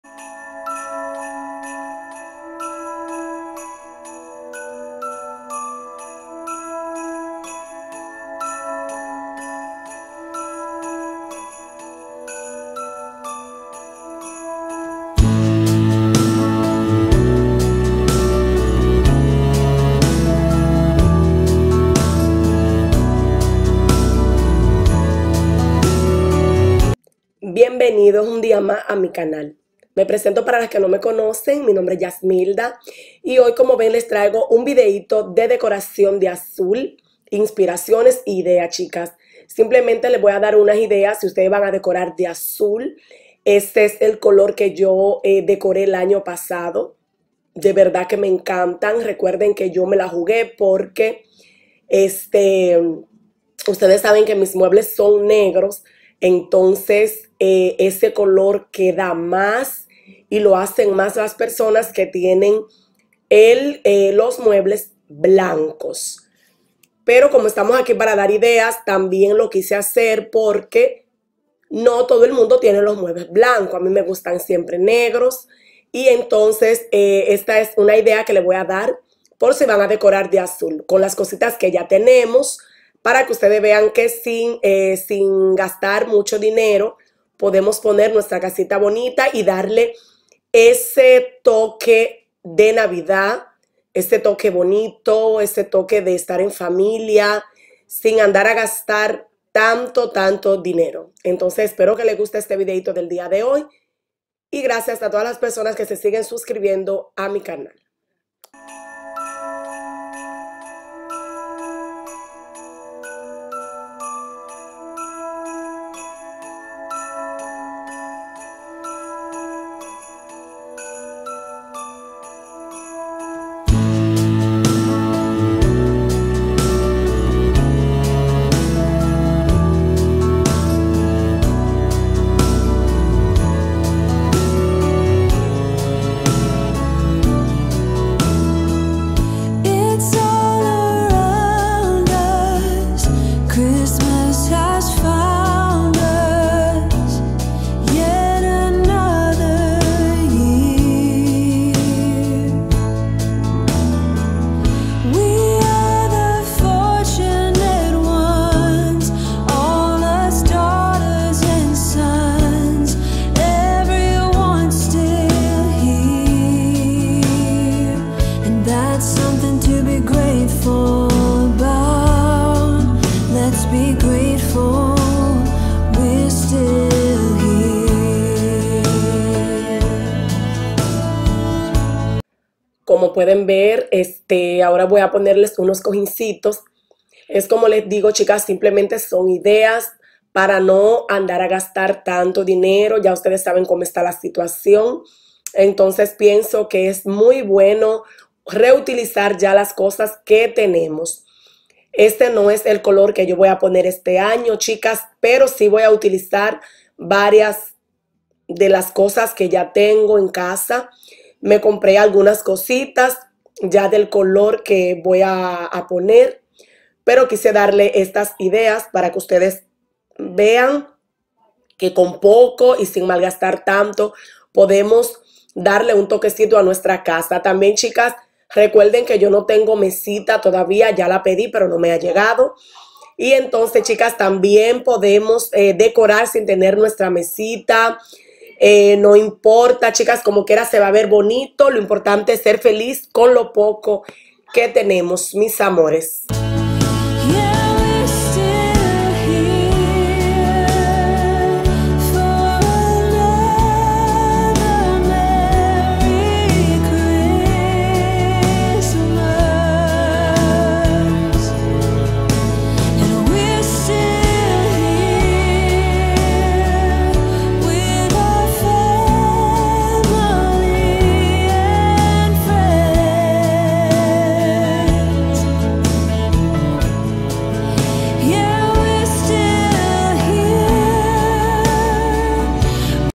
DECORACION 2020 IDEAS DE NAVIDAD EN AZUL
HOLA MIS BELLAS EL DIA DE HOY LES COMPARTO IDEAS  NAVIDEÑA PARA LA SALA ESPERO QUE LES GUSTE

instagram jamybyglam